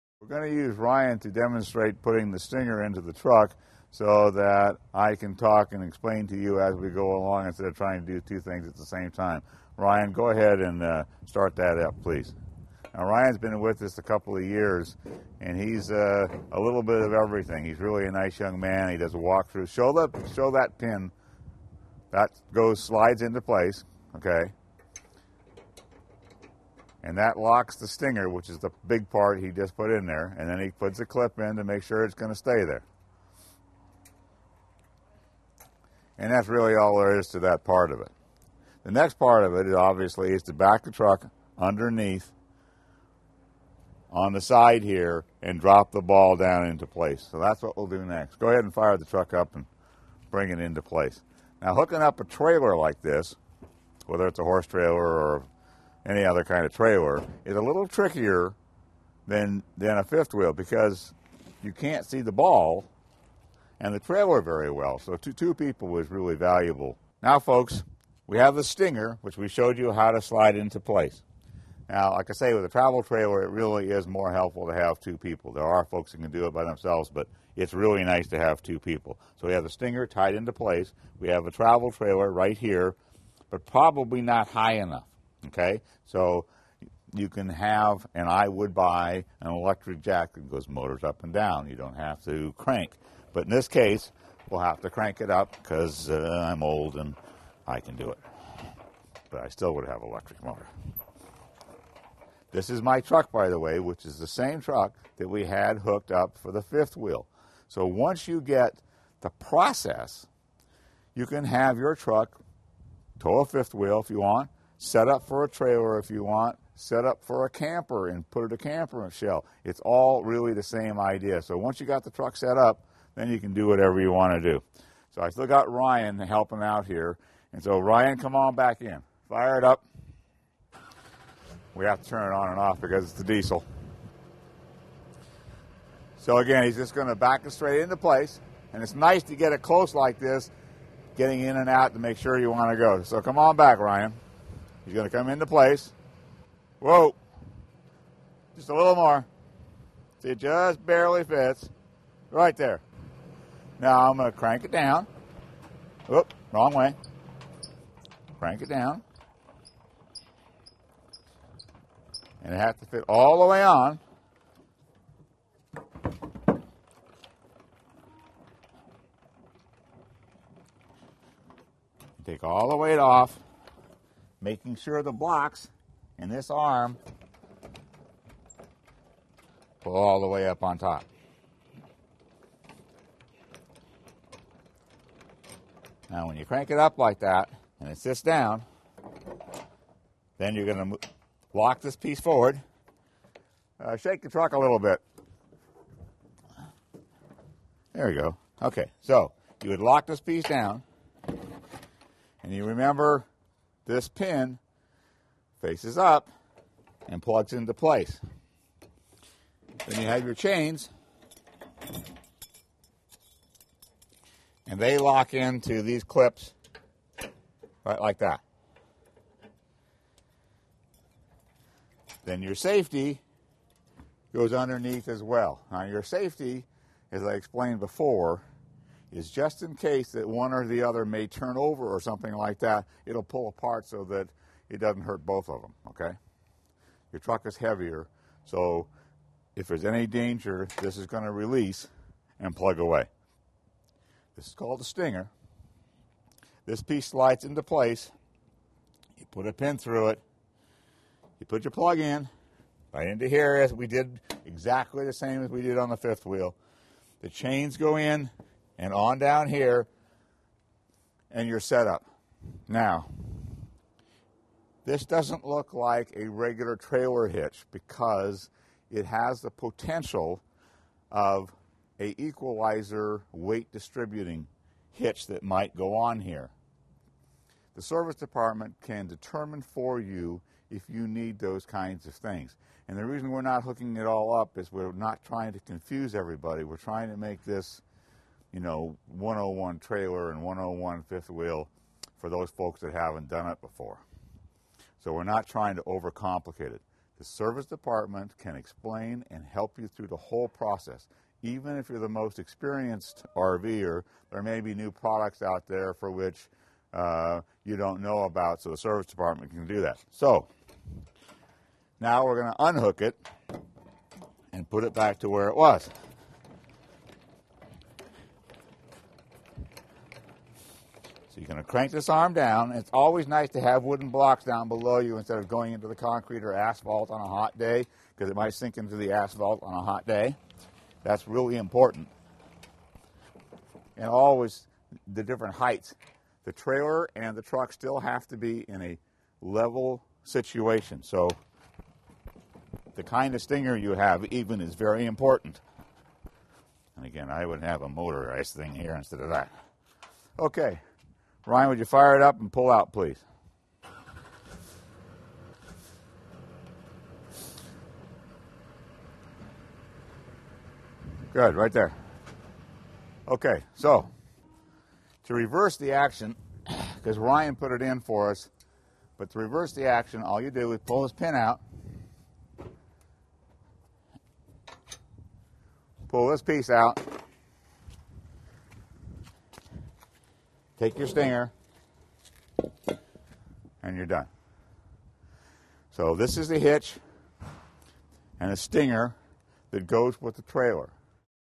Travel Trailer Hookup
A real-life travel trailer hook-up video demonstration of locking in a hitch for a travel trailer or 5th wheel. Our RV service department also offers instruction and demos.